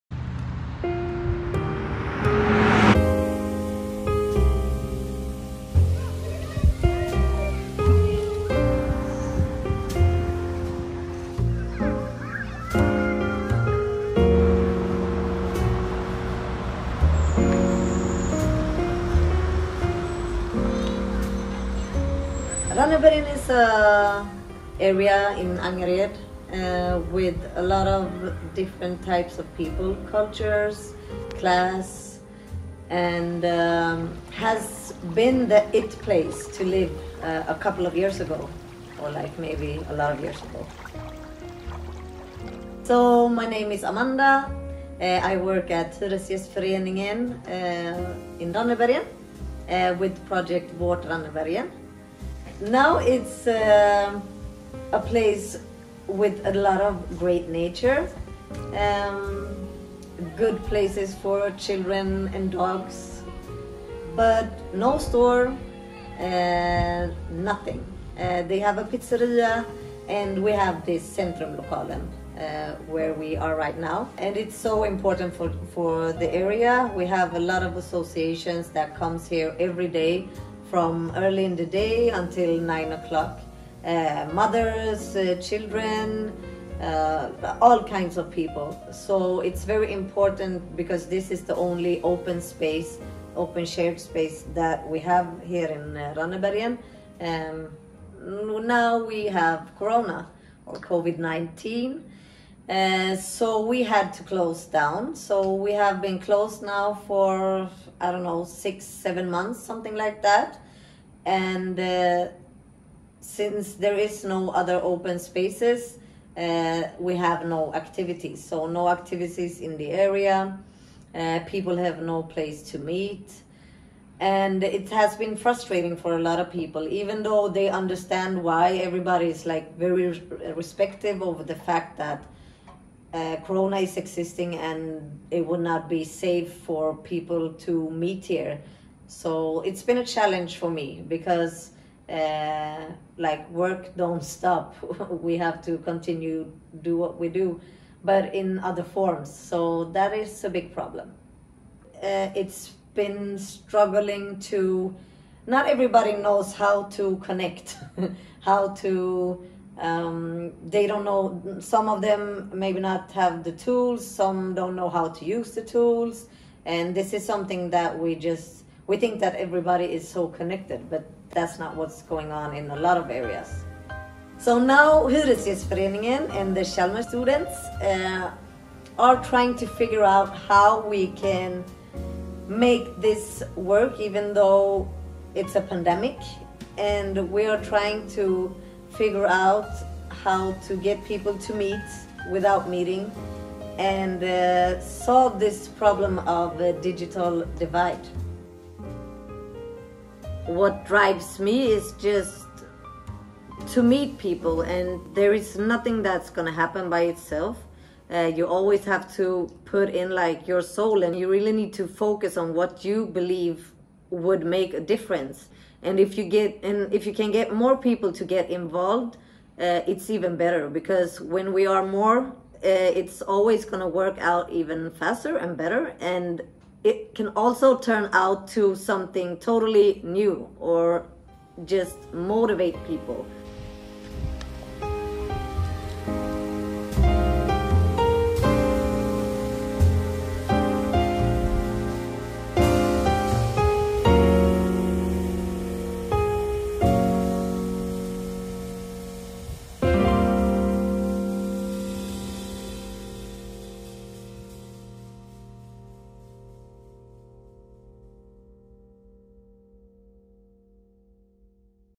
Meeting Rannebergen - Chalmers Social Inclusion 2020
In Rannebergen, the important local community meeting space - where the residents used to have daily activities - has been closed since spring due to Covid 19. Amanda, the project leader of ”Our Rannebergen” at the Tenant Association, is calling out for help to bring the residents together again; could it be by connecting the residents through digital tools? However, not everyone has the tools or knows how to use them. How can we bridge the digital divide?

A documentary made as an assignment for the course "Design and Planning for Social Inclusion 2020" made by students of the Chalmers University of Architecture.